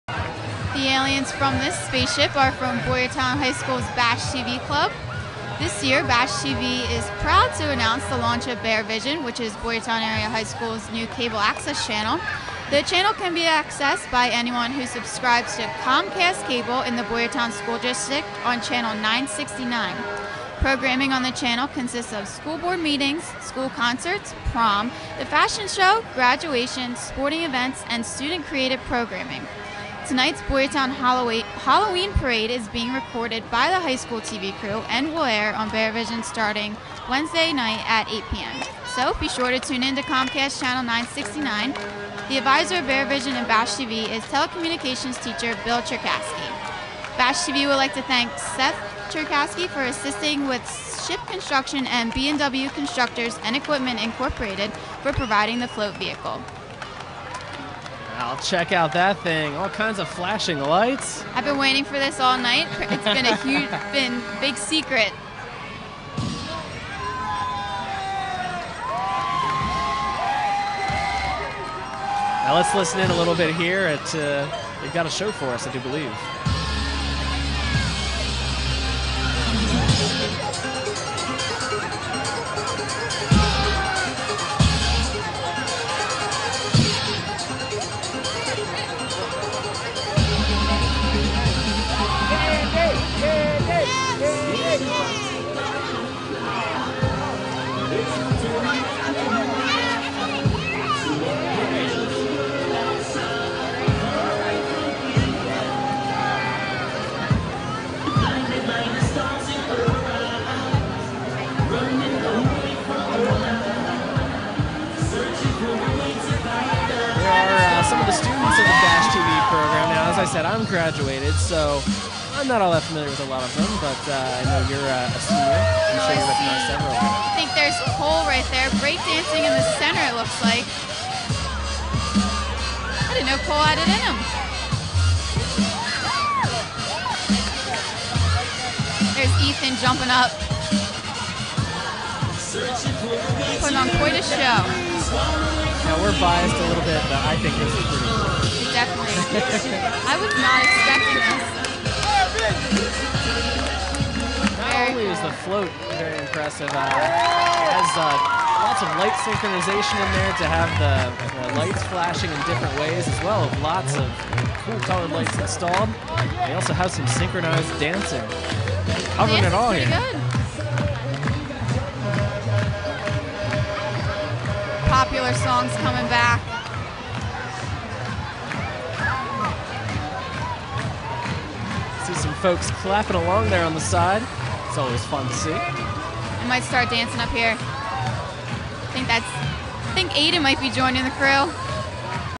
BASH-TV/BASD-TV Float in Boyertown Halloween Parade
Video of the BASH-TV/BASD-TV float in the 60th Annual Boyertown Halloween Parade on October 20th, 2012.  The float was to promote the launch of Boyertown School District's new cable access channel, BASD-TV Bearvision.